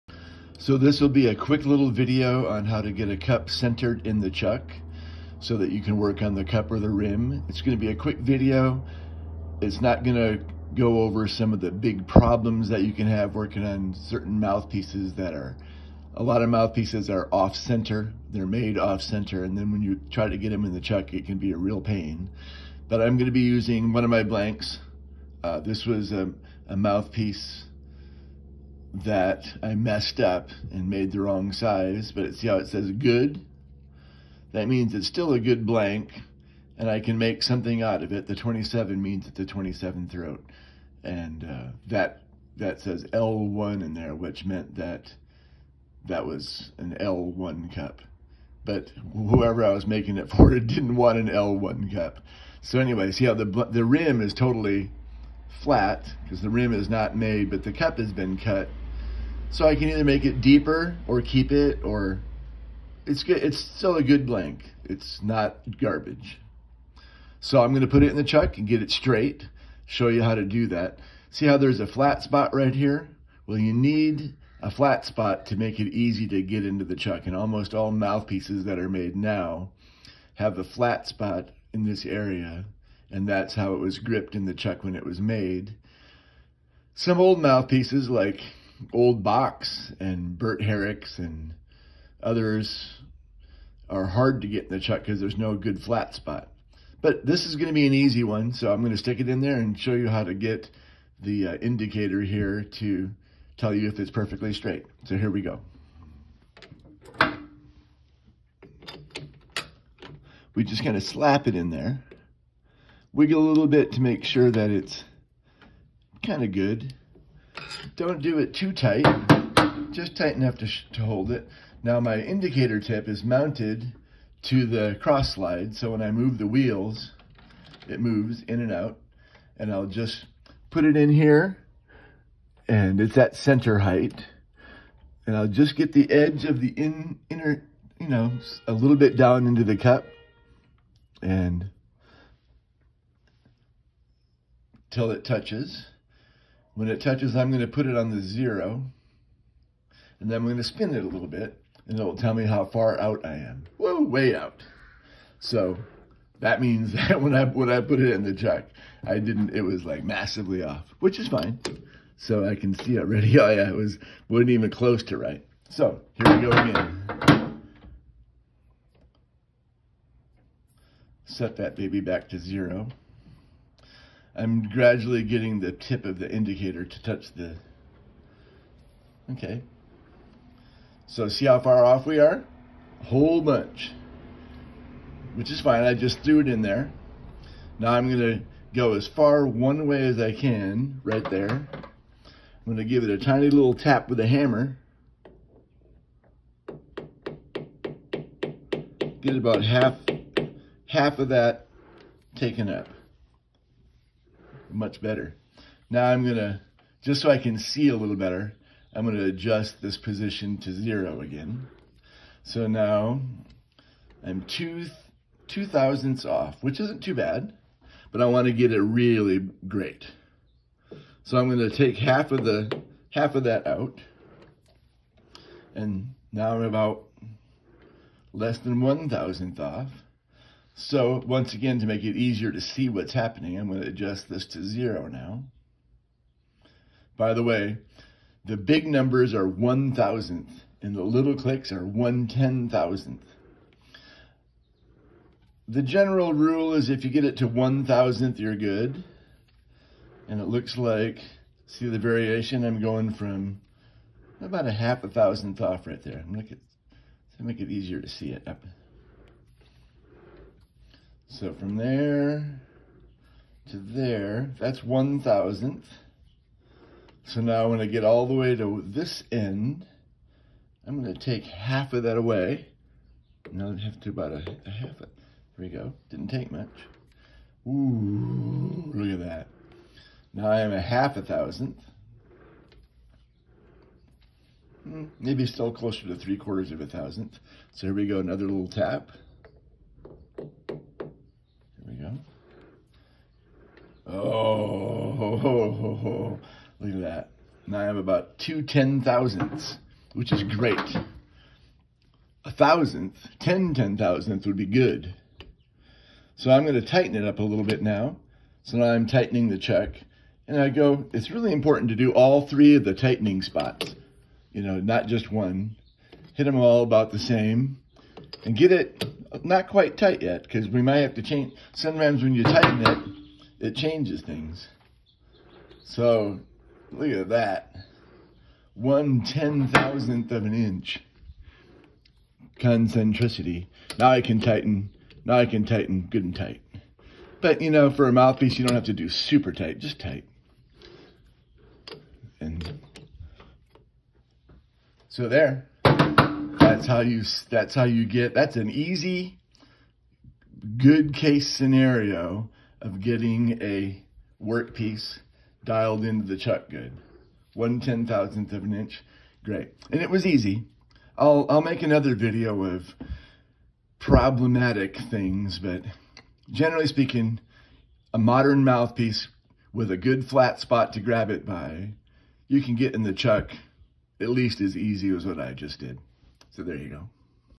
TUTORIAL: Centering a trumpet mouthpiece in the chuck
This is for centering a trumpet mouthpiece in a lathe chuck, and getting it straight. Somemouthpiece are hard to center if they dont have a "flat spot" to hold onto, or if the mouthpiece was made with the inside off center to the outside etc. In those instances you can still do it, but its more complicated. I'll do another video for situations like that